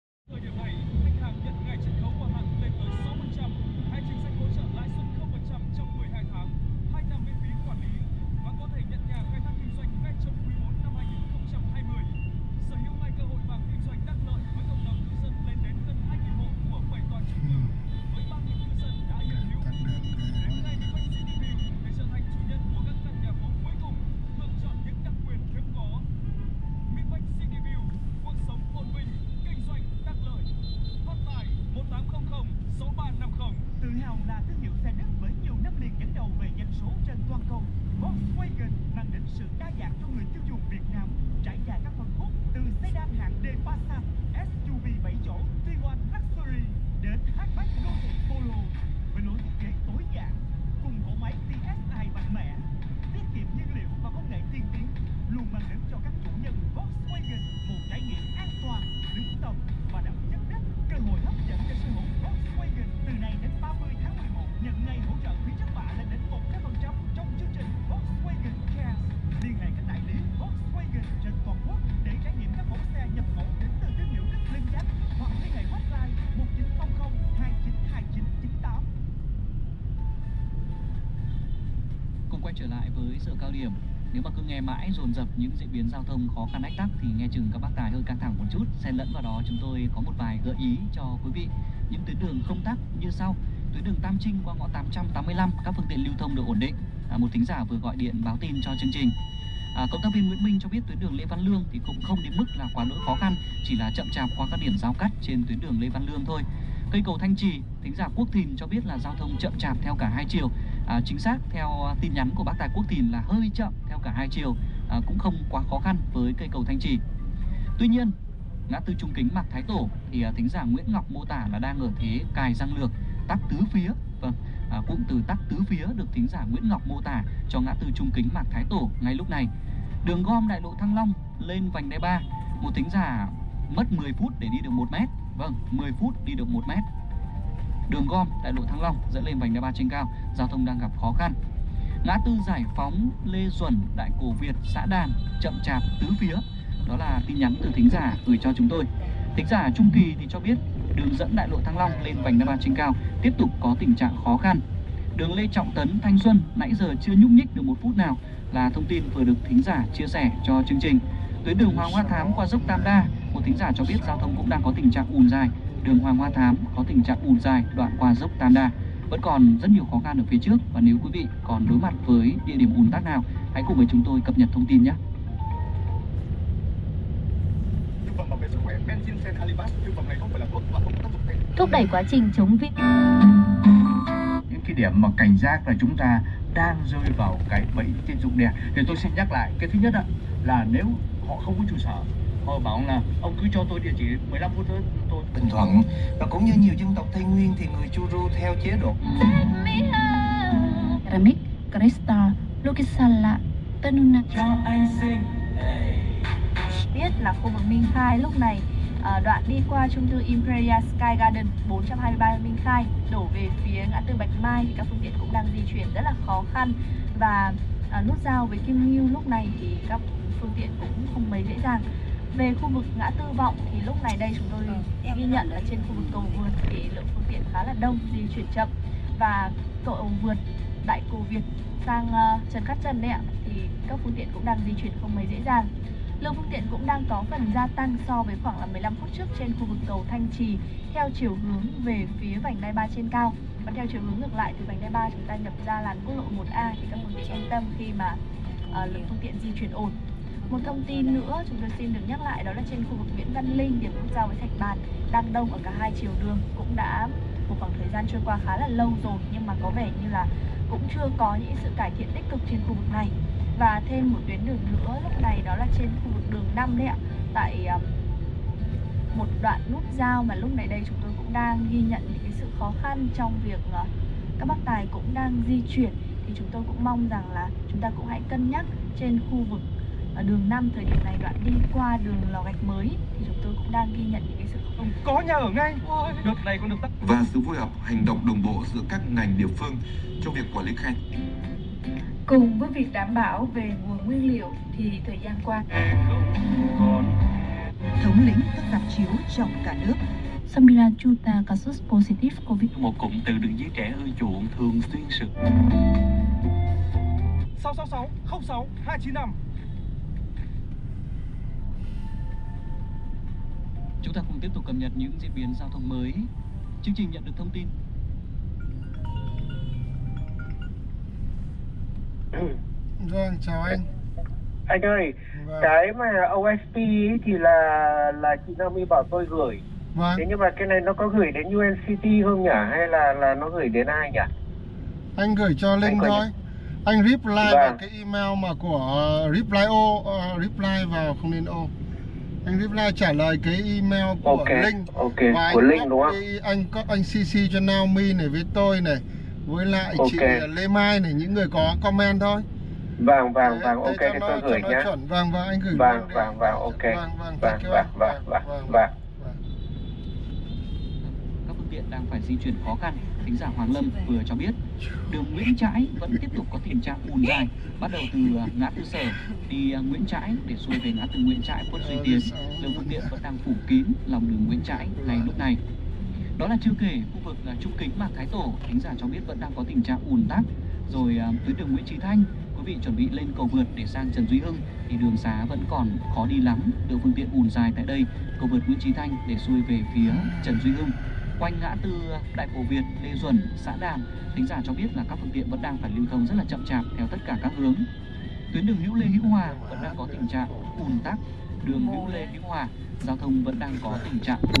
ĐƯỜNG HÀ NỘI THÁNG 11 NĂM 2020 (THỜI COVID)
CAMERA VỀ CUỘC SỐNG ĐÃ BÌNH THƯỜNG Ở VIỆT NAM,
(NHIỀU NƯỚC ĐẠI DỊCH COVID VẪN CÒN!!)
NXHYT/NOV. 2020